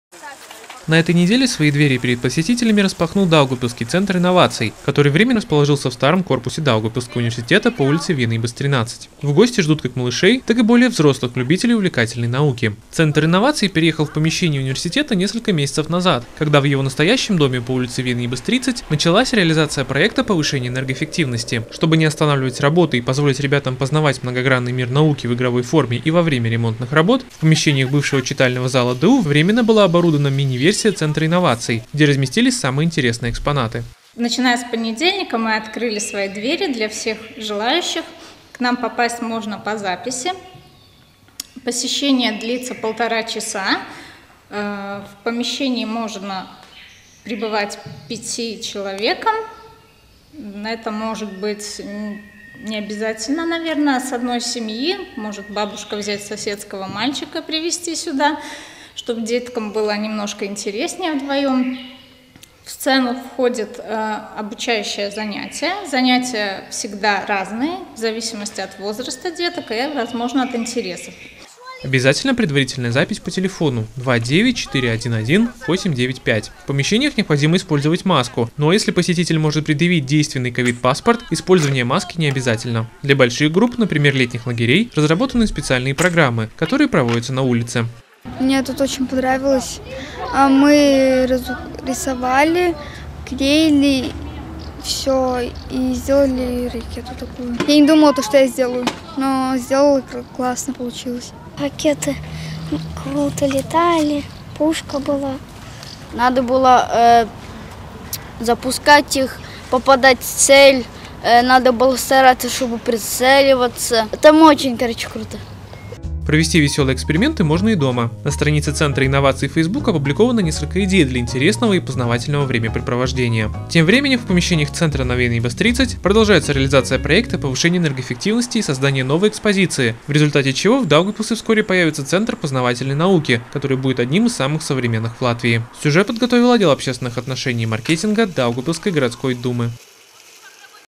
Центр инноваций ждет посетителей в «старом» корпусе ДУ
На этой неделе двери перед посетителями распахнул Даугавпилсский Центр инноваций, который временно расположился в «старом» корпусе Даугавпилсского Университета по ул.Виенибас, 13. В гости ждут как малышей, так и более взрослых любителей увлекательной науки.
Центр инноваций переехал в помещения Университета несколько месяцев назад, когда в его «настоящем доме» по ул.Виенибас, 30 началась реализация проекта повышения энергоэффективности. Чтобы не останавливать работу и позволить ребятам и во время реализации проекта продолжать познавать в игровой форме многогранный мир науки, в помещениях бывшего читального зала ДУ временно была оборудована «мини-версия» Центра инноваций, где разместились самые интересные экспонаты.